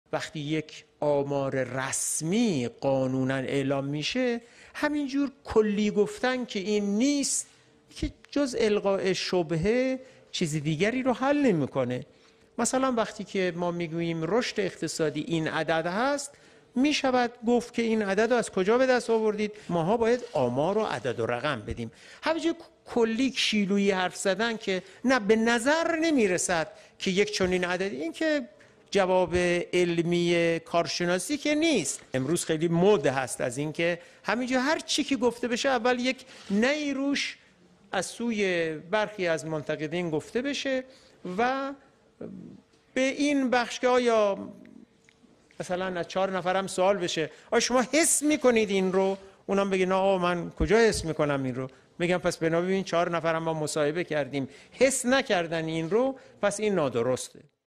نوبخت: منتقدان کیلویی حرف می‌ زنند!
امروز برخی منتقدین کیلویی آمار رسمی را زیر سوال می‌برند/ از ۴ نفر سوال می‌پرسند چیزی حس می‌کنید؟